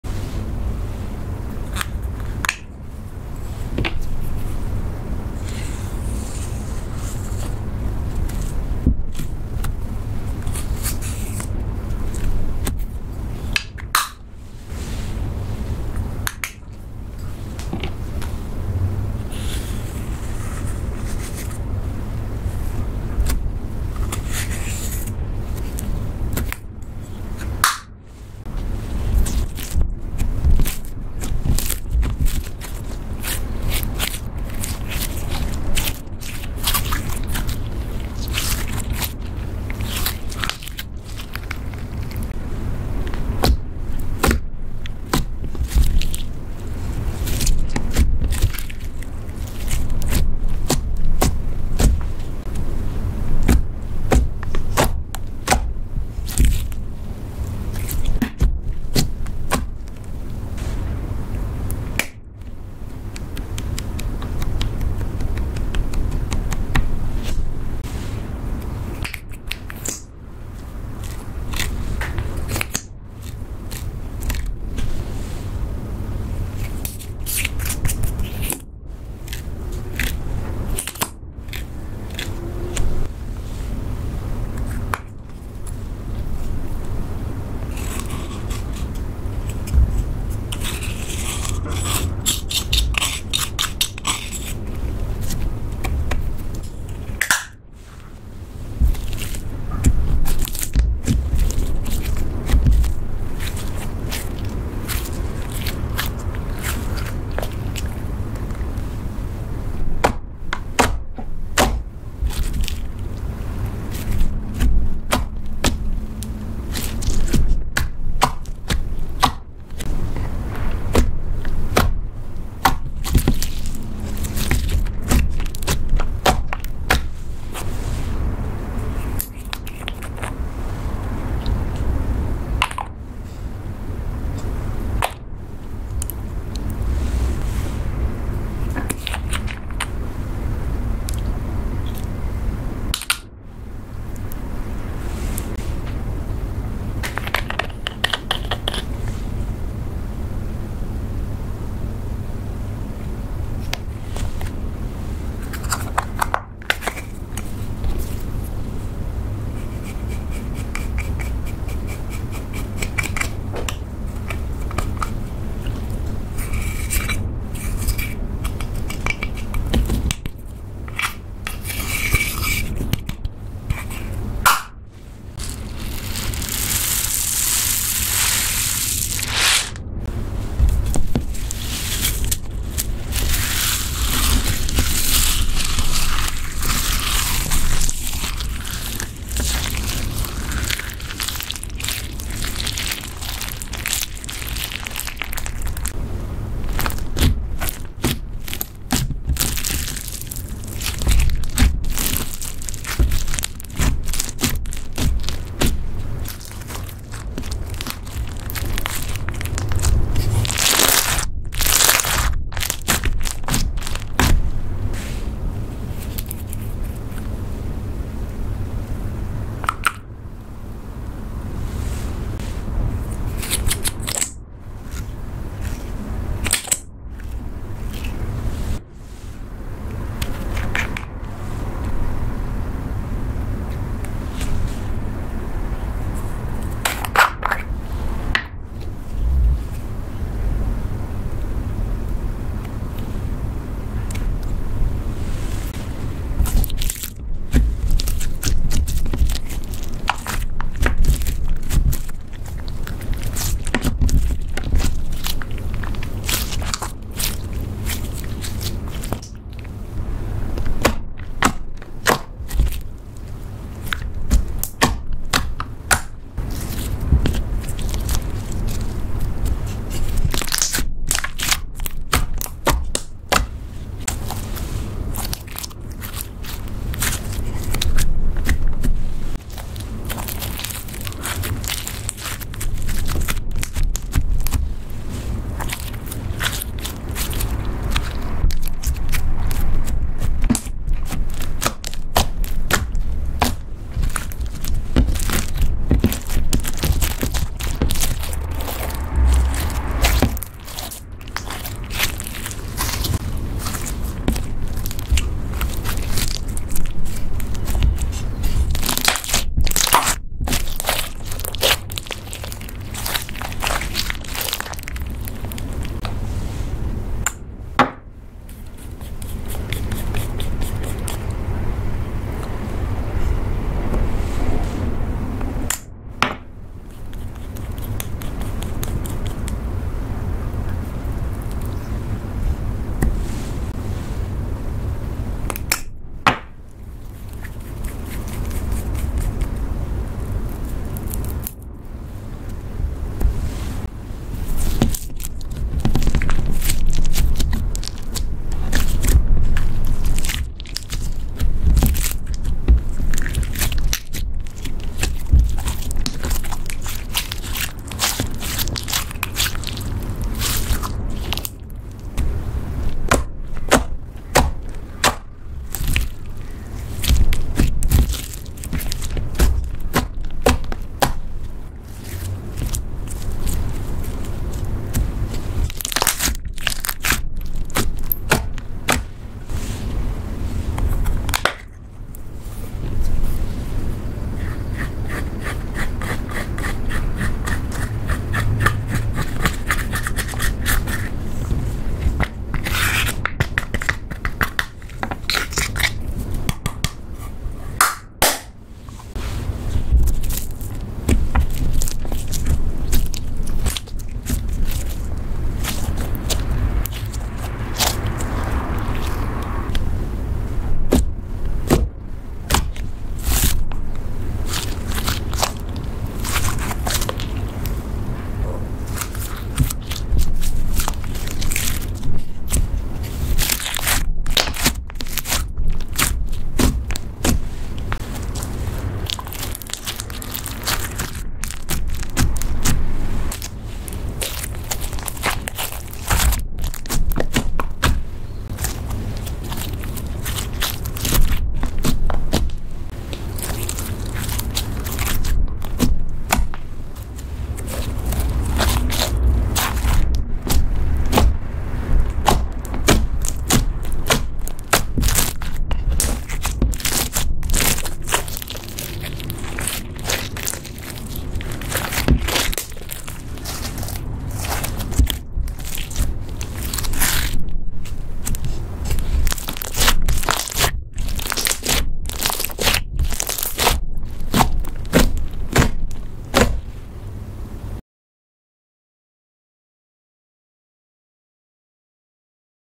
Satisfying Slime Coloring with Makeup! Mixing Eyeshadow & Lipstick into Clear Slime! #1
Hi Guys😋✨

Today's video is Mixing Eyeshadow & Lipstick into Clear Slime 😚💖